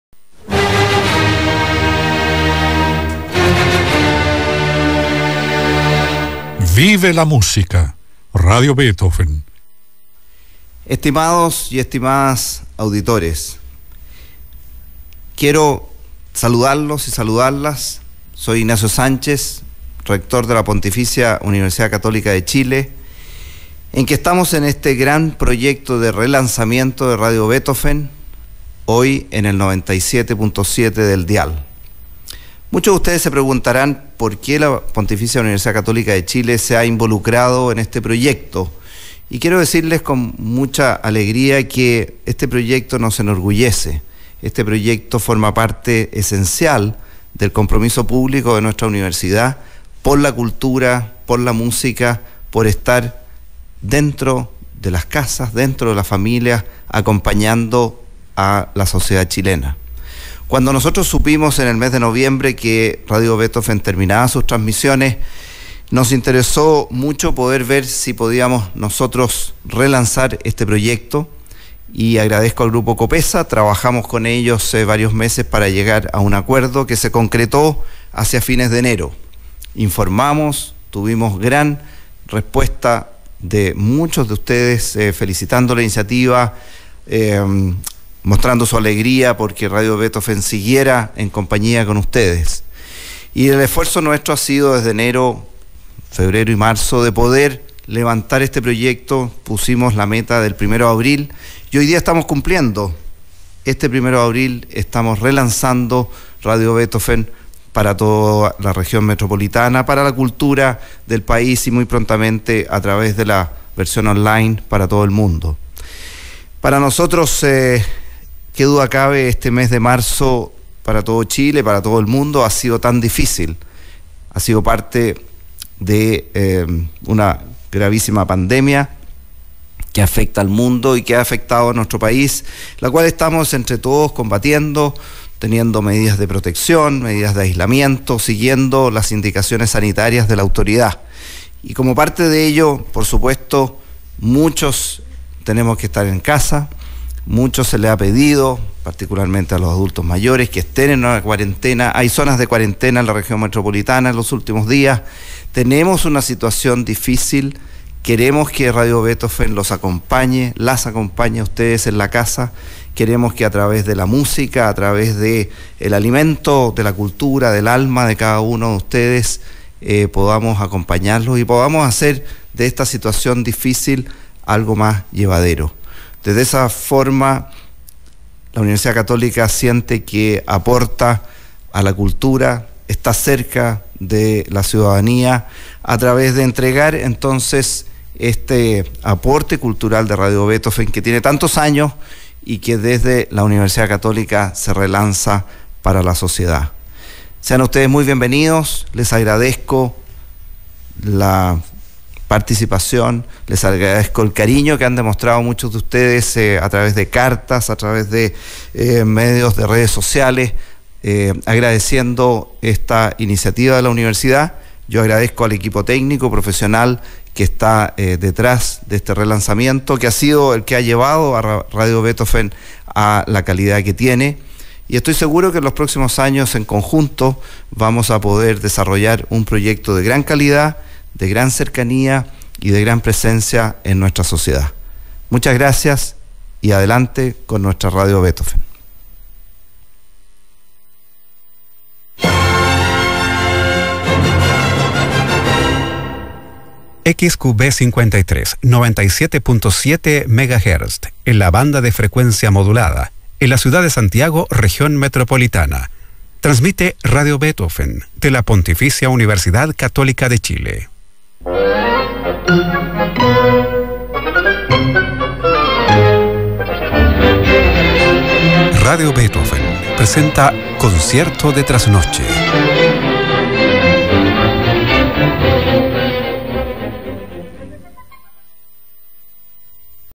Relanzamiento radio Beethoven 97.7 FM 1 de Abril de 2020
Inicio de transmisiones de radio Beethoven con discurso del Rector de la Universidad Católica Ignacio Sánchez, para el relanzamiento de la radio, el 1 de Abril de 2020.